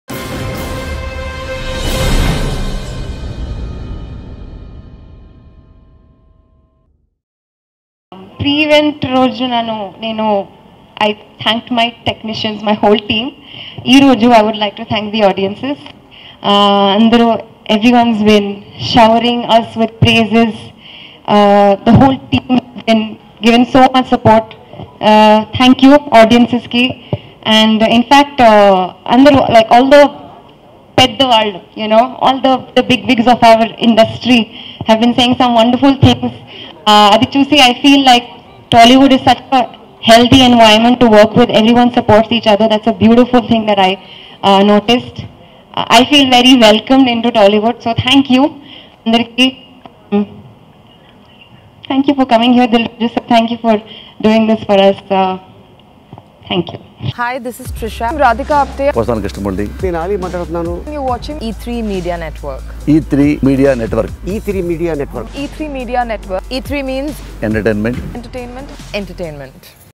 Shraddha Srinath Lovely Speech | JERSEY Appreciation Meet | Nani, Gowtham | E3 Talkies
Watch Shraddha Srinath Lovely Speech at JERSEY Appreciation Meet
Jersey Movie Starring Nani and Shraddha Srinath directed by arjun, Music Composed by Anirudh